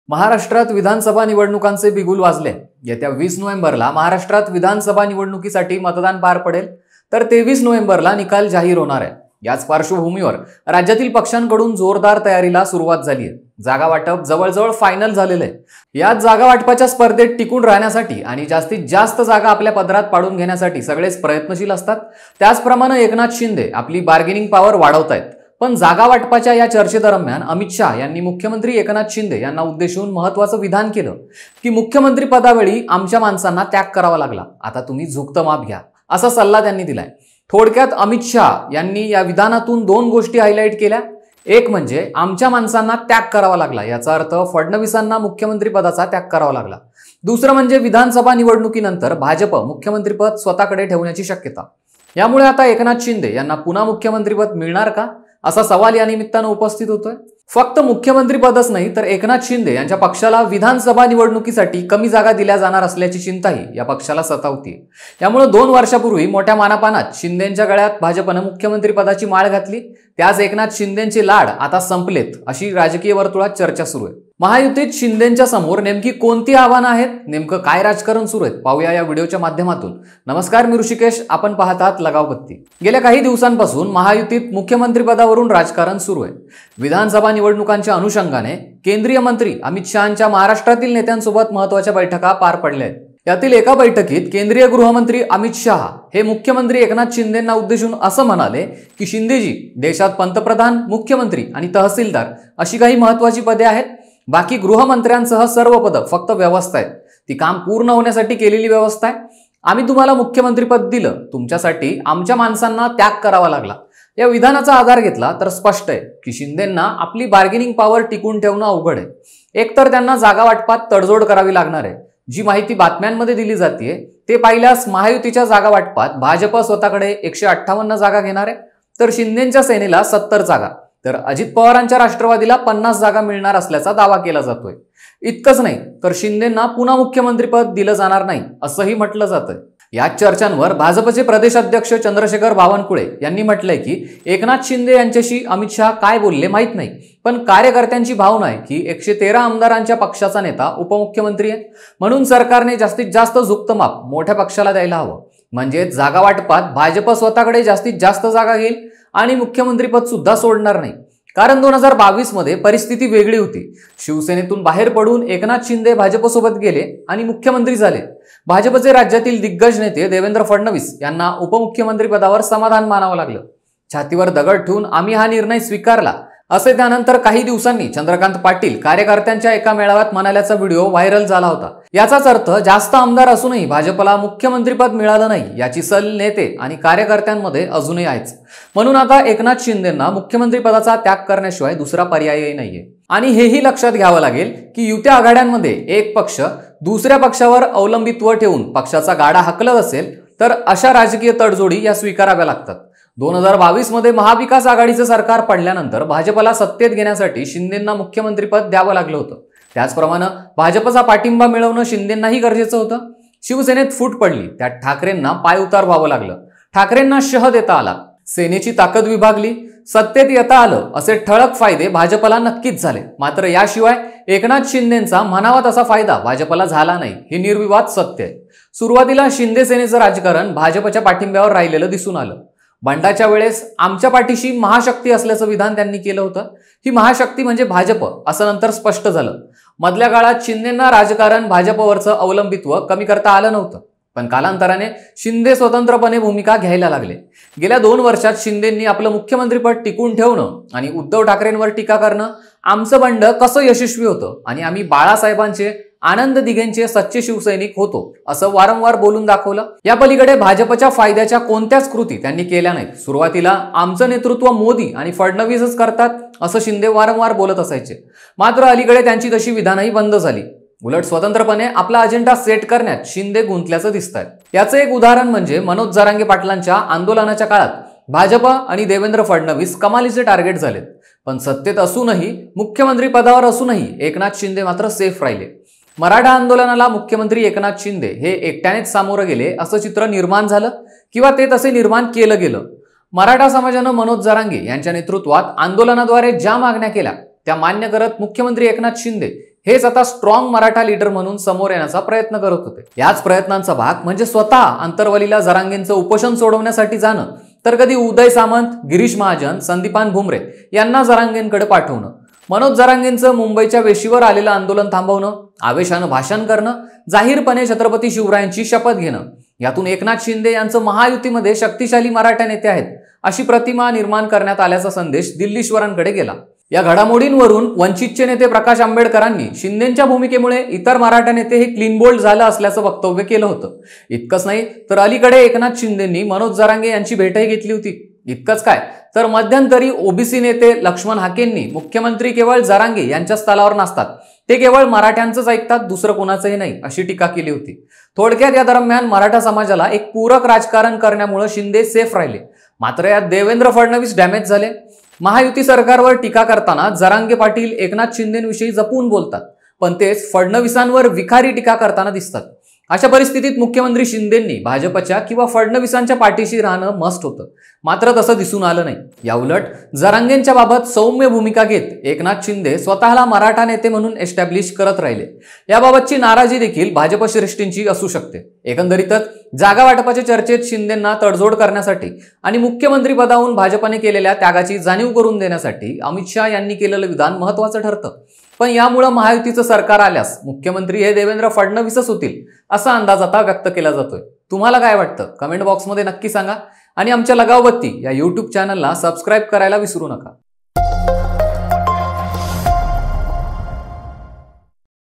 Eknath Shinde चे मानपान संपले ? अमित शहांनी स्पष्ट शब्दांत शिंदेना काय सांगितलं ?
महाराष्ट्रात विधानसभा निवडणुकांचे बिगुल वाजले आहे. येत्या 20 नोव्हेंबरला महाराष्ट्रात विधानसभा निवडणुकीसाठी मतदान पार पडेल. तर 23 नोव्हेंबरला निकाल जाहीर होणार आहे. याच पार्श्वभूमीवर राज्यातील पक्षांकडून जोरदार तयारीला सुरुवात झाली आहे. जागावाटप जवळ जवळ फायनल झालेलं आहे. याच जागावाटपाच्या स्पर्धेत टिकून राहण्यासाठी आणि जास्तीत जास्त आपल्या पदरात पाडून घेण्यासाठी सगळे प्रयत्नशील असतात त्याचप्रमाणे एकनाथ शिंदे आपली बार्गेनिंग पॉवर वाढवत आहेत पण जागा वाटपाच्या याच चर्चेदरम्यान अमित शहा यांनी मुख्यमंत्री एकनाथ शिंदे यांना उद्देशून महत्त्वाचं विधान केलयं, त्यामुळं दोन वर्षांपूर्वी मोठ्या मानपानात शिंदेंच्या गळ्यात भाजपने मुख्यमंत्री पदाची माळ घातली त्याच एकनाथ शिंदेंचे लाड आता संपलेत अशी चर्चा राजकीय वर्तुळात सुरूय. महायुतीत शिंदेंच्या समोर कोणती आव्हानं आहेत ? नेमकं काय राजकारण सुरुये पहा या व्हिडिओच्या माध्यमातून.

🛑आमच्या चॅनेलला आजच सबस्क्राईब करा🛑